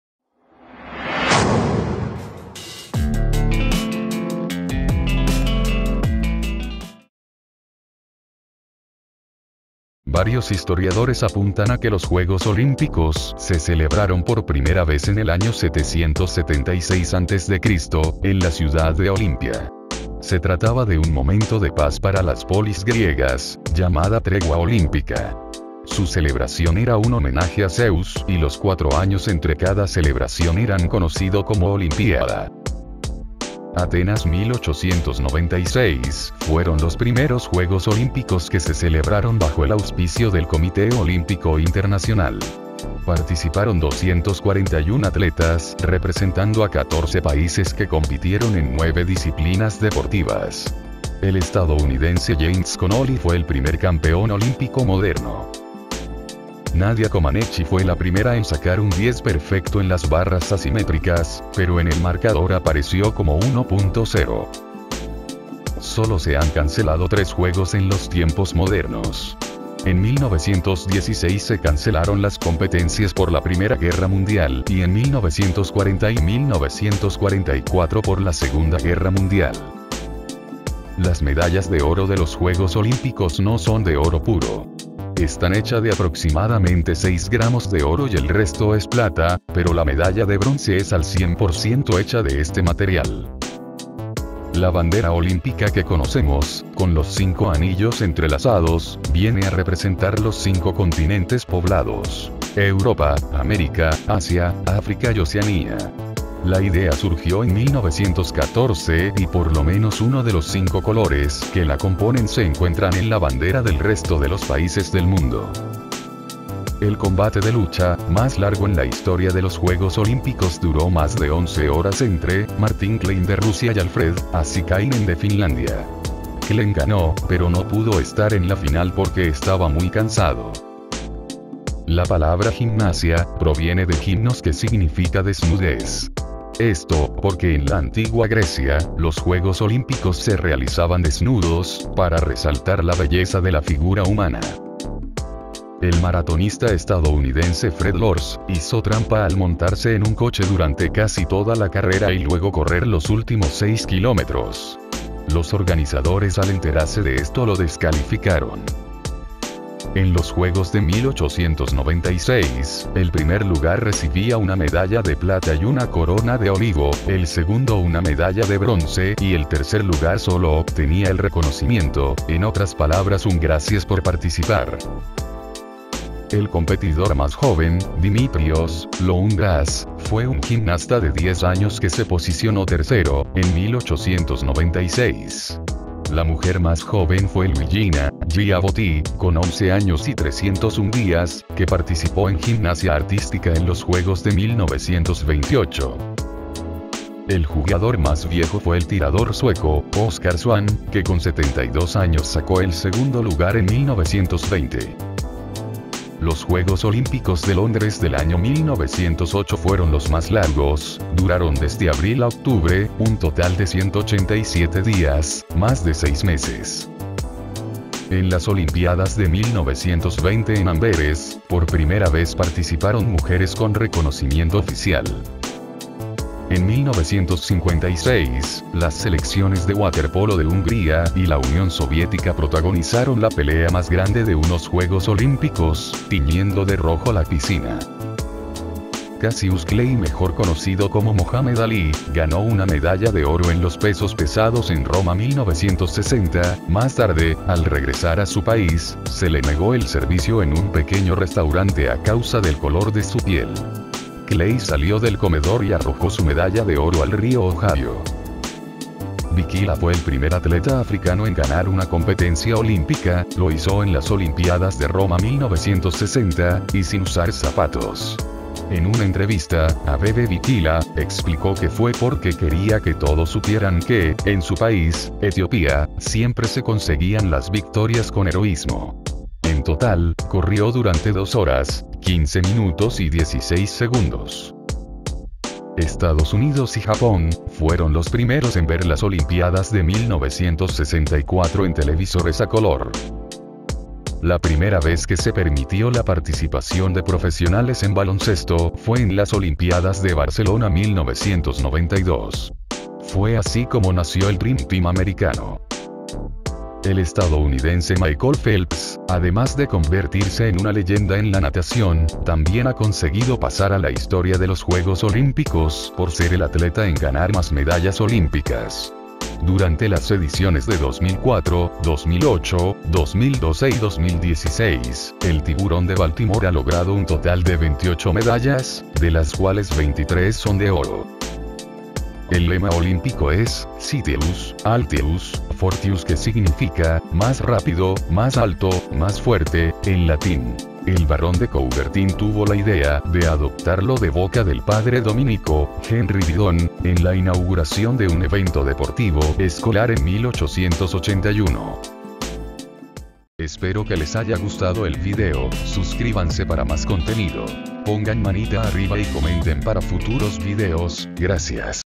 Curiosidades de los Juegos Olímpicos
Uno de los eventos deportivos más importantes que reúne a los mejores atletas del mundo para competir en diferentes disciplinas deportivas y demostrar de una vez por todas quien es el mejor. Los juegos Olímpicos a través de los años se han enriquecido con curiosidades y hechos poco comunes que aderezan más la justa Olímpica. En este video veremos unas curiosidades que han ocurrido en los Juego Olímpicos.